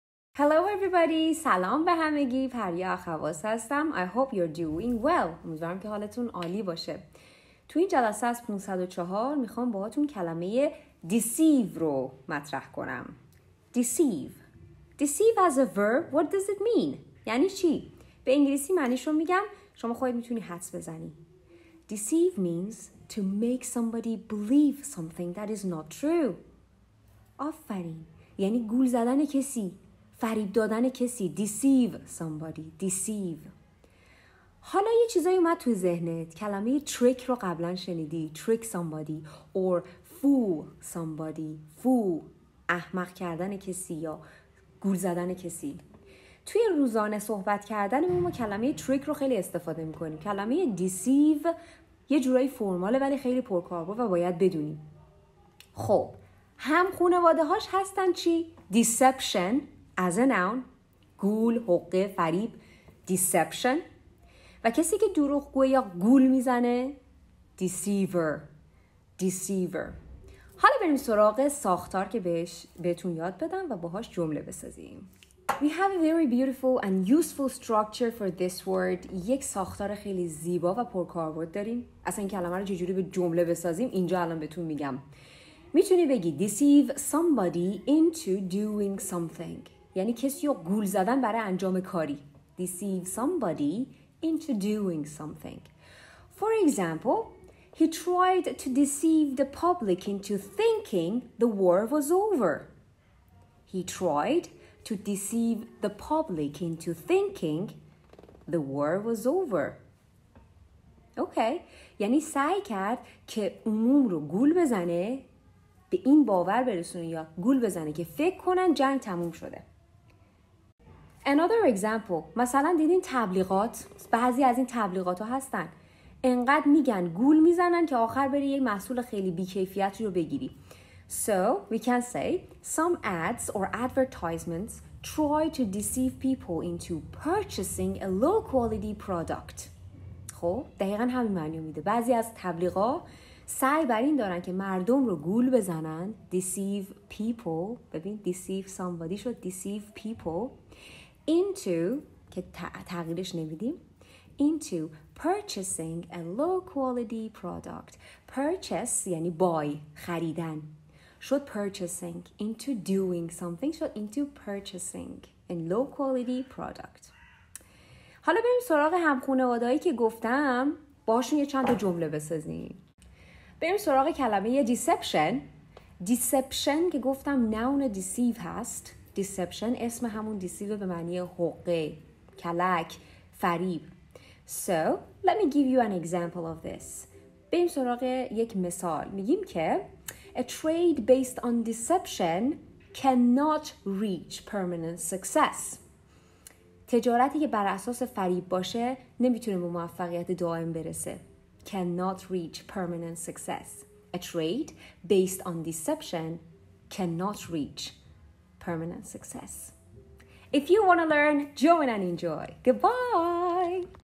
۵۰۴ کلمه ضروری انگلیسی با پریا | deceive |کلمات و لغات لازم برای انگلیسی پیشرفته
کلمه  Deceive از مجموعه 504 واژه ضروری انگلیسی
504 Absolutely Essential Words
این مجموعه شامل آموزش ۵۰۴ واژه ضروری انگلیسی که از جمله پرکاربرد ترین کلمات در امتحانات آیلتس تافل و GRE می باشند

مدرس پریا اخواص